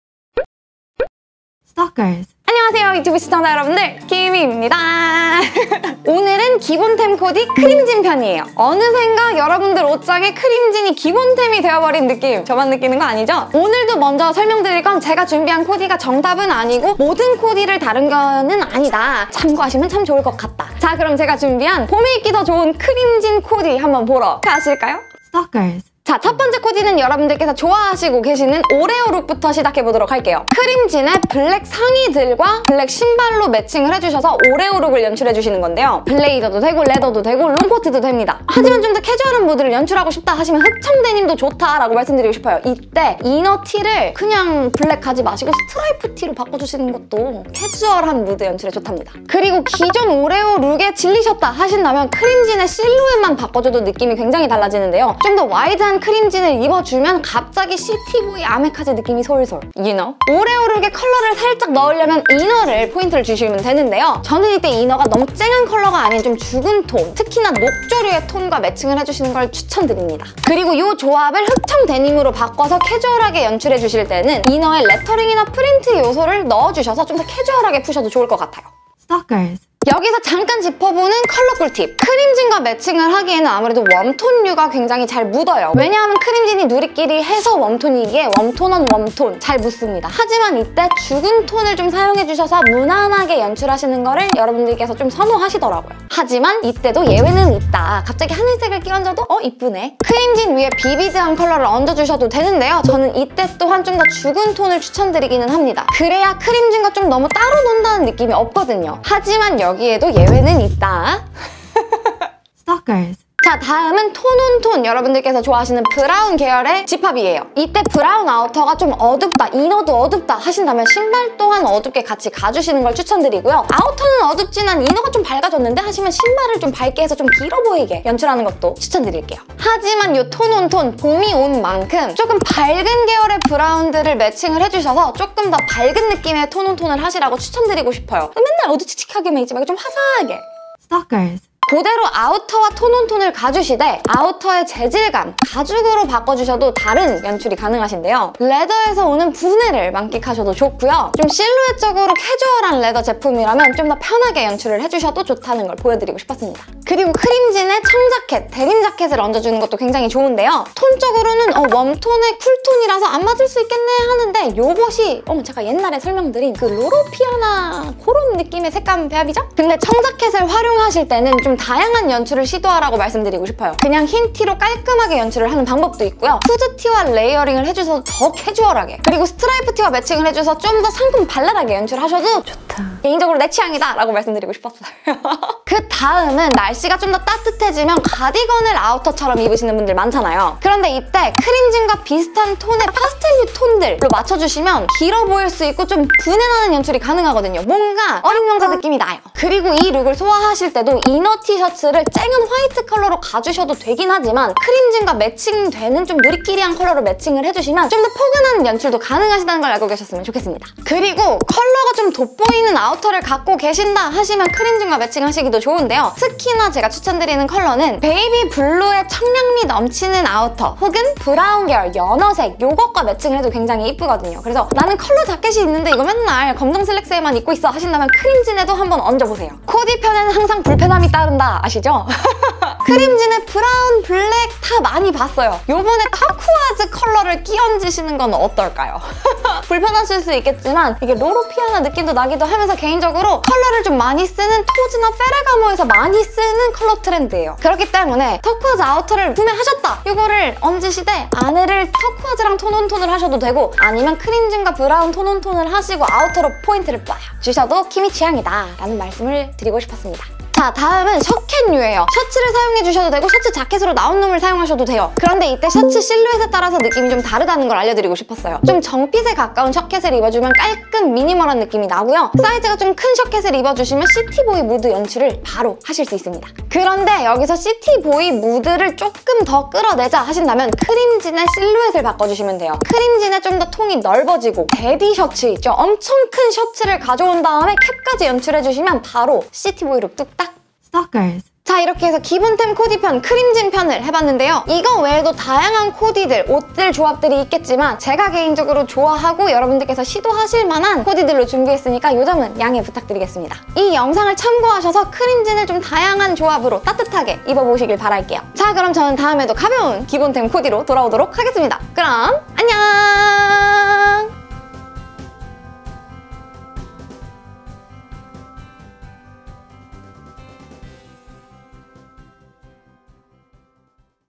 [기본템 코디] 크림진 코디하기
오늘은 기본템 코디편 제 5탄!!
크림진 코디입니다!

이제는 크림진도 기본템이라고 생각할만큼
많은 분들의 사랑을 받는 아이템인데요!

크림진의 대표 코디인 블랙 아우터에
크림진을 매치해서 입으셔도 예쁘지만
오늘은 여러분의 옷장에도 입을 법한 제품들로 코디를 준비해봤으니
제가 준비한 코디를 참고하셔서 다양한 크림진 코디해보셨으면 좋겠습니다!!

[브랜디드] 51007 HISHITOMO NATURAL CREAM JEANS [RELAX STRAIGHT] -모델 30 사이즈 착용

[세터] Vinales - Four Tuck Six Pocket Wide Jean - 모델 30(M) 사이즈 착용

[에스파이벗] 카펜터스 퍼티그 치노팬츠(IVORY) - 모델 S 사이즈 착용

0:00 오프닝
0:25 오레오룩
1:19 크림진 위에 컬러 얹기
1:52 톤온톤
2:20 아우터 얹기
4:51 엔딩